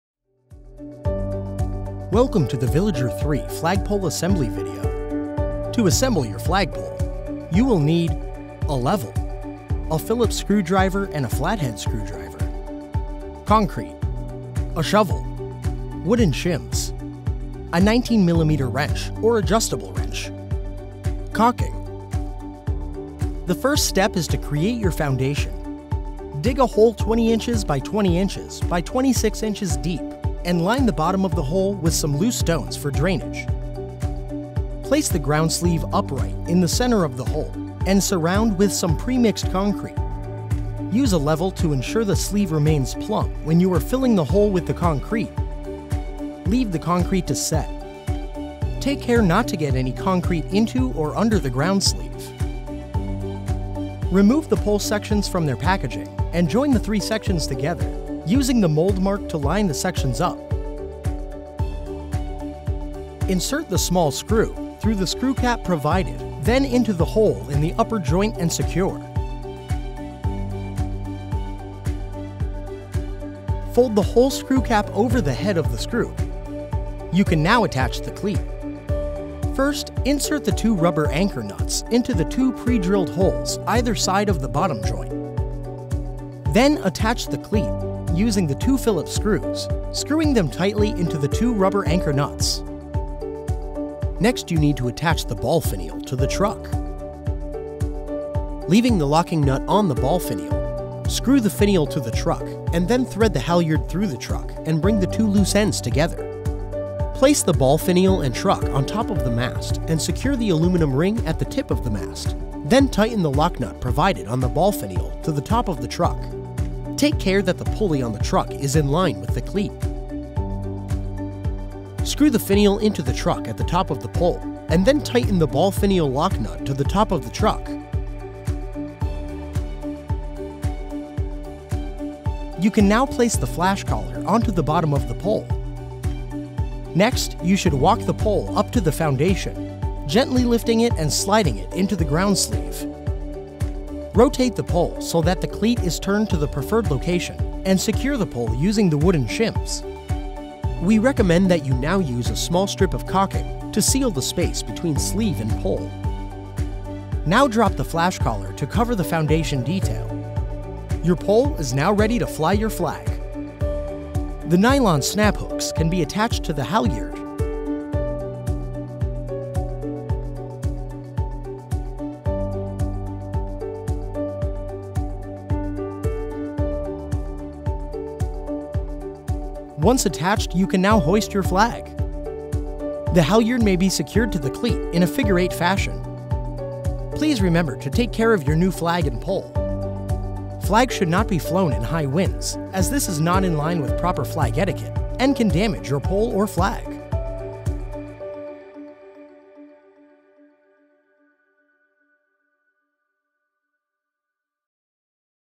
Annin Villager III Flagpole
Installation instructions for the 2016 Villager Flagpole models 395 and 3952